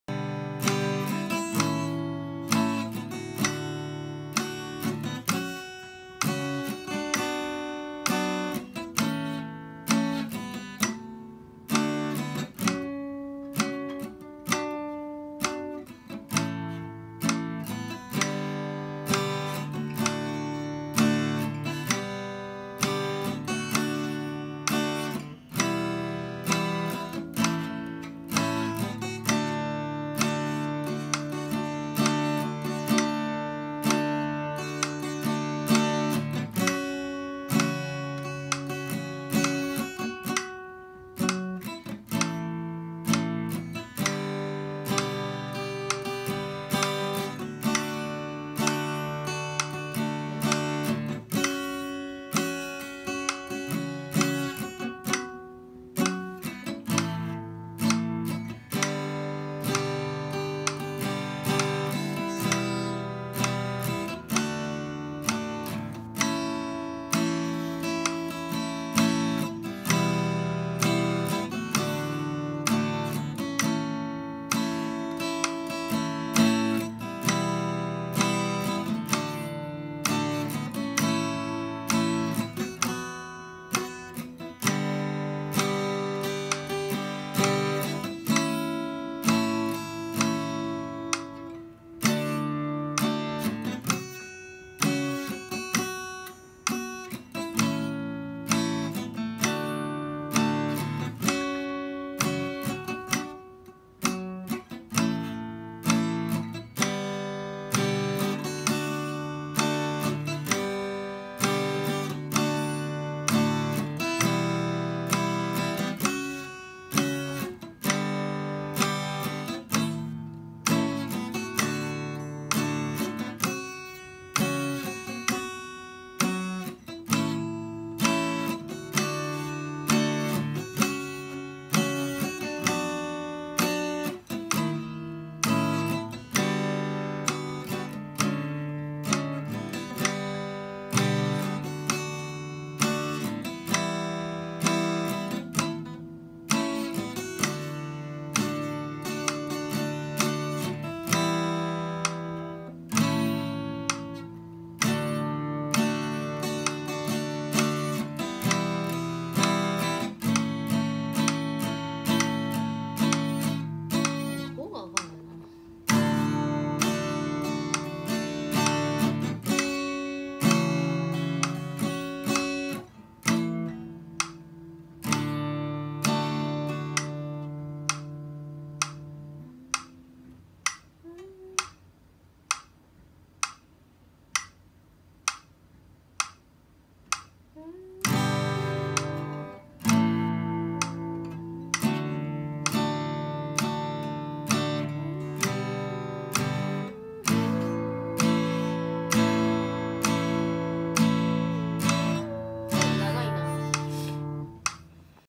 【kinoko39】 【ギターの練習】 メトロノームを使って 練習をする柴田愛実（まなみ） （大阪市大正区）（2019/7/22） （koumeican） （martin 00015M）
【kinoko39】 【ギターの練習】
メトロノームを使って
練習をする柴田愛実（まなみ）
（大阪市大正区）（2019/7/22）
（singer song writer）
（koumeican channel）
（martin  00015M）